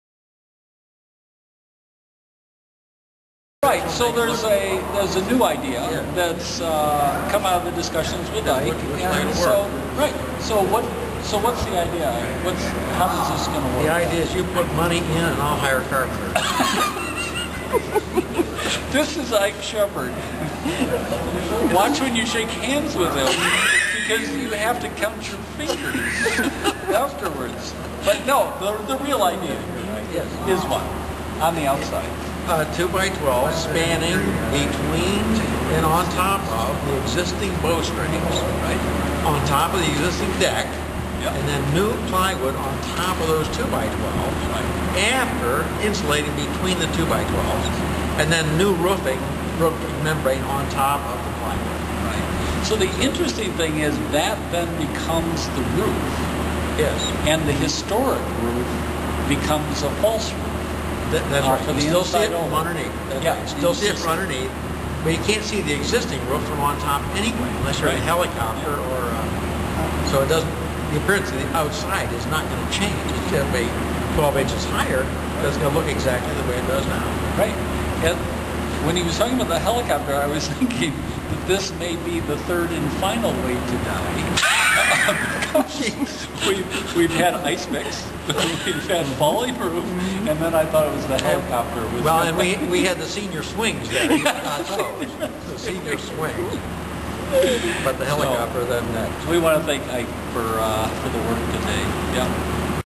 I Like Ike
Meeting with Ike Shepherd, structural engineer, and Tom Brennan, on January 18, 2008, to address the structural issues with the roof of the Green Garage (4444 Second Ave, Detroit).